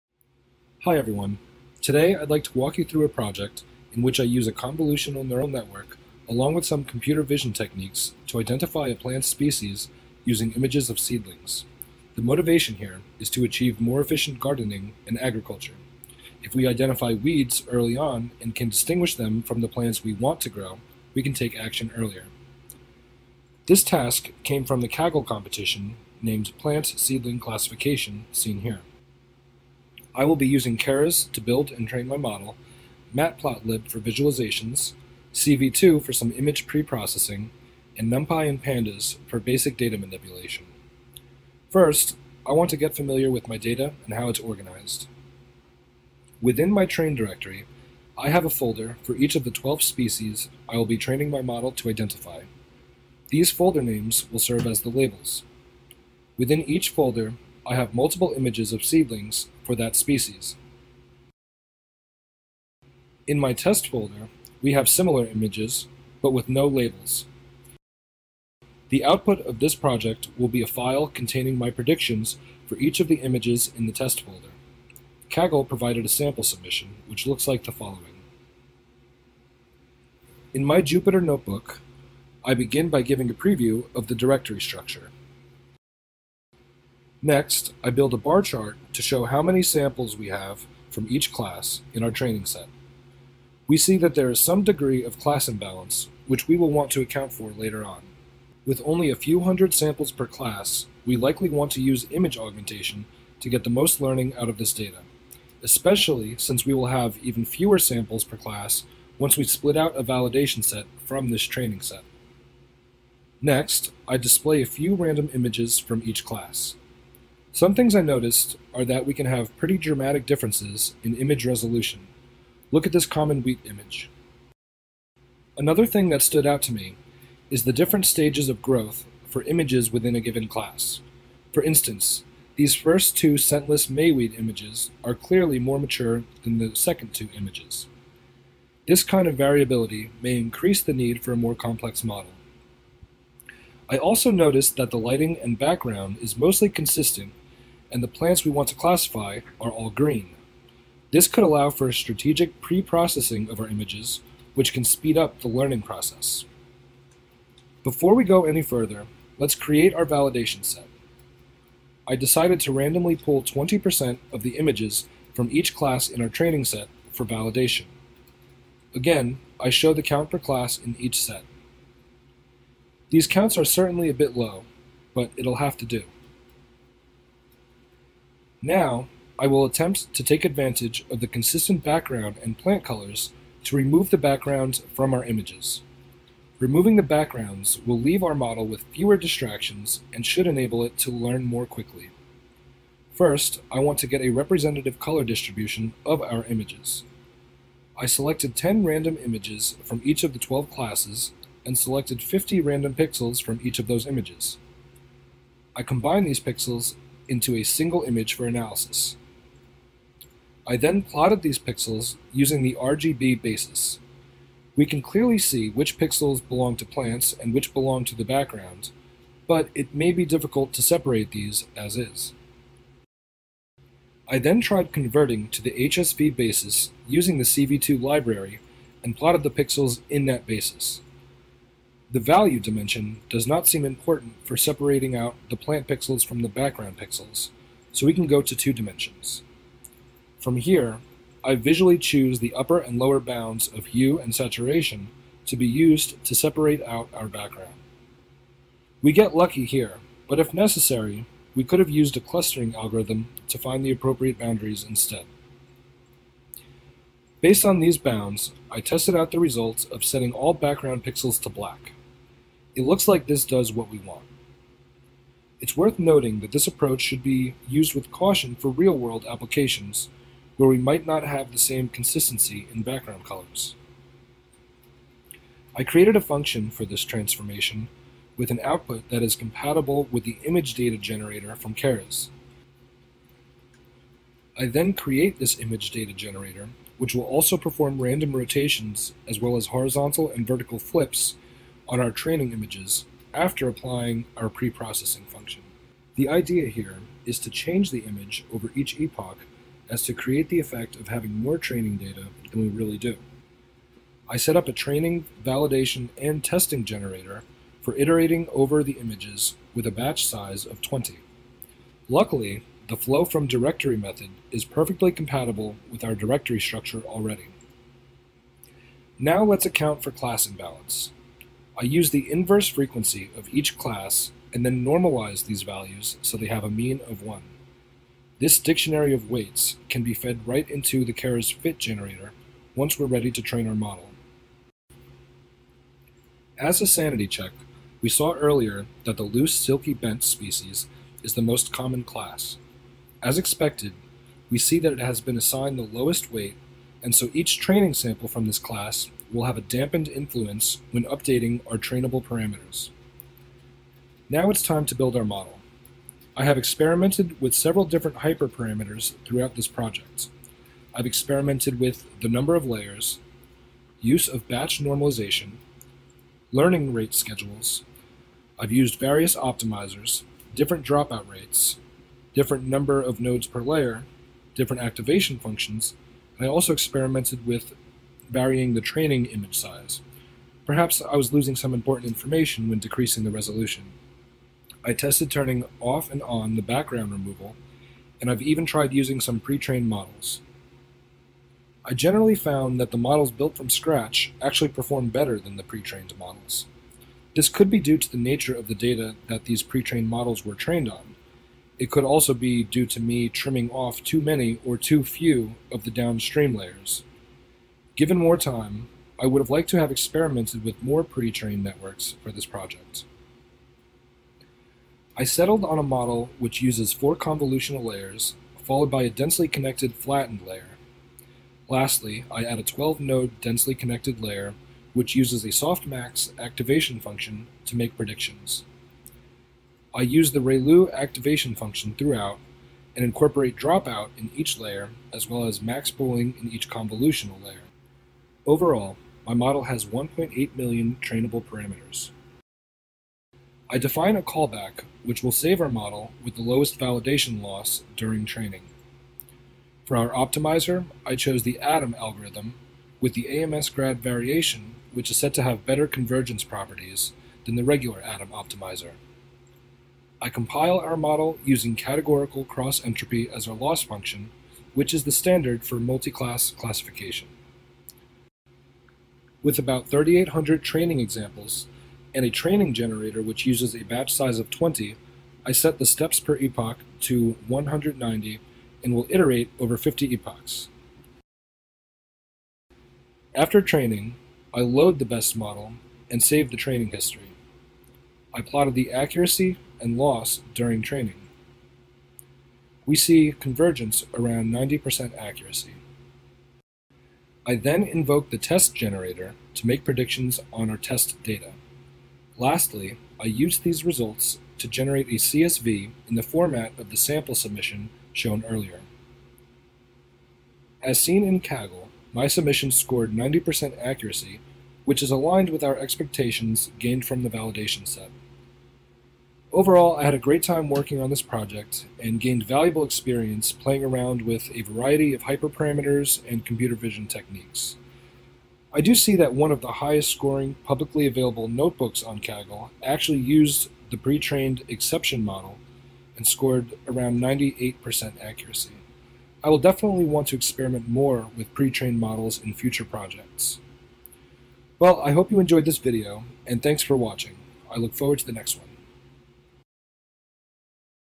Deep Learning For Plant Classification
Deep dive into a project in which I use Keras to build a deep learning model for classifying plant species based on images of seedlings.